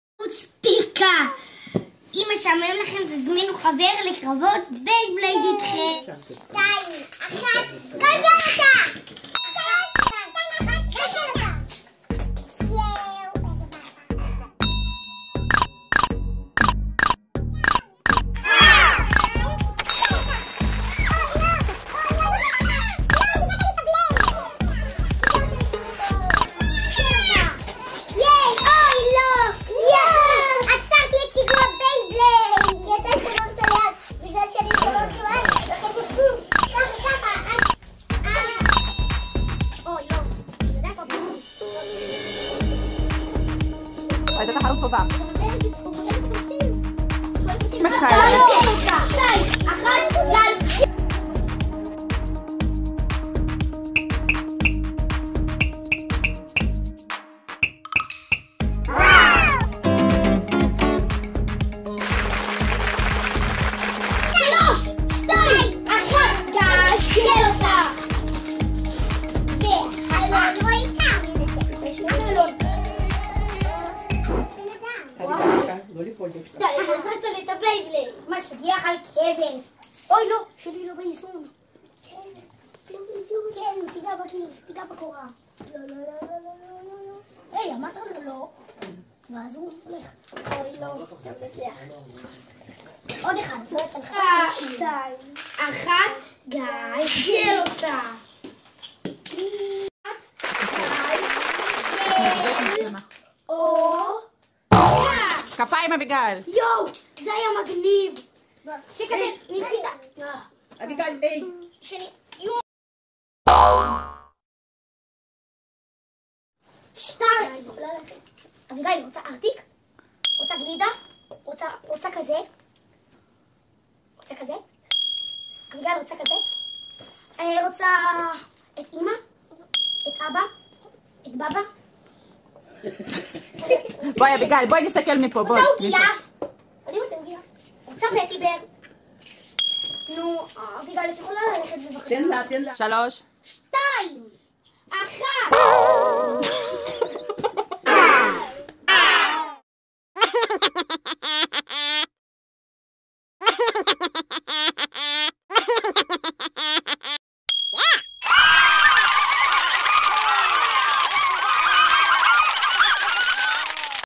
קרב בייבלייד עם חבר או אח. דברים מגניבים שאפשר לעשות בבית בעקבות הנחיות קורונה. משעמם לי פרק 2
WithMe     Stay Home#
קרבות בייבלייד מרתון קרבות של התנפצות . מה עושים כשנשארים יותר מידי זמן בבית בגלל הקורונה.
רעיונות לפעילות לילדים בבית בגלל הנחיות קורונה
מה עושים כשמשעמם לי?

מזמינים חבר ומשחקים בקרבות בייבלייד אפילו כשאחיות קצת מפריעות.